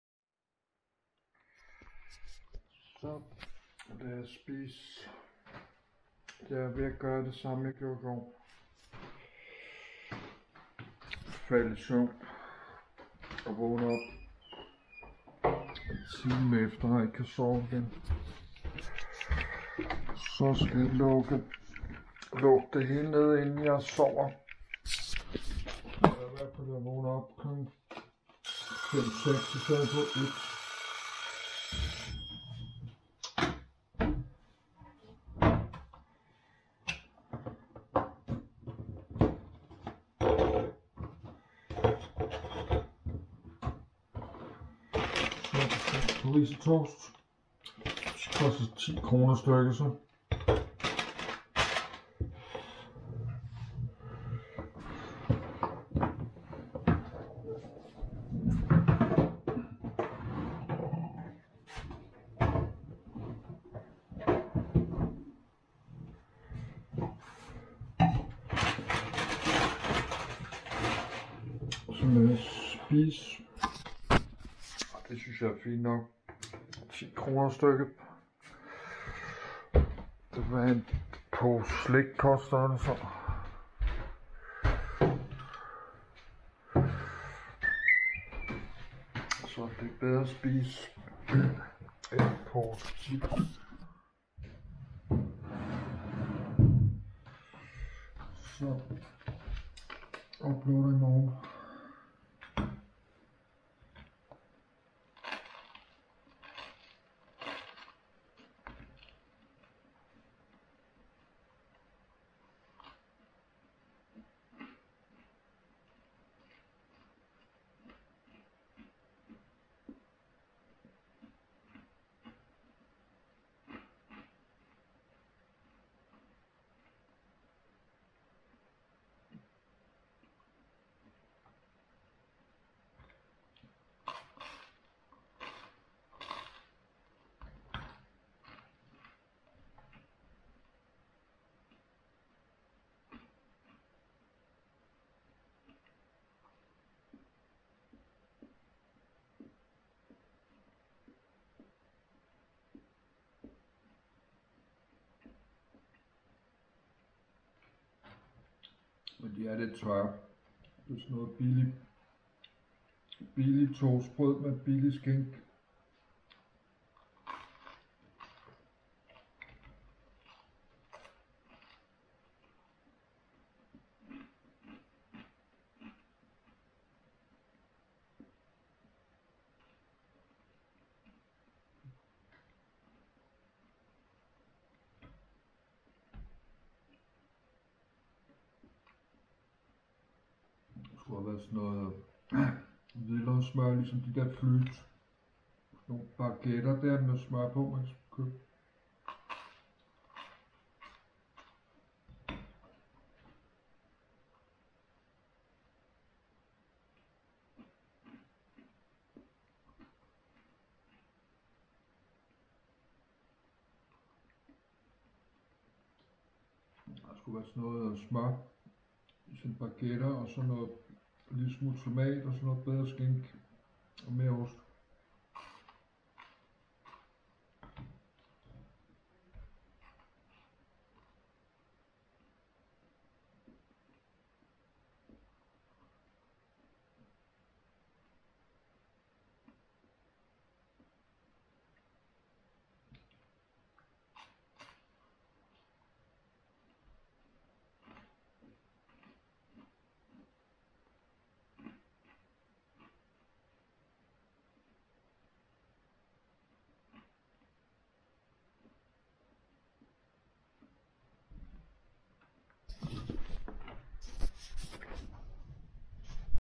MongoTV_15211 - Min Natmad - Del 2 - PARISER TOAST
DEL 2 - Godmorgen, og god TIRSDAG. Jeg spiste lige, 2 frost toastbørd, sent mandag midnat. Så optog jeg lige, de her 2 video. Det er tirsdag nu, og klokken den er snart 9.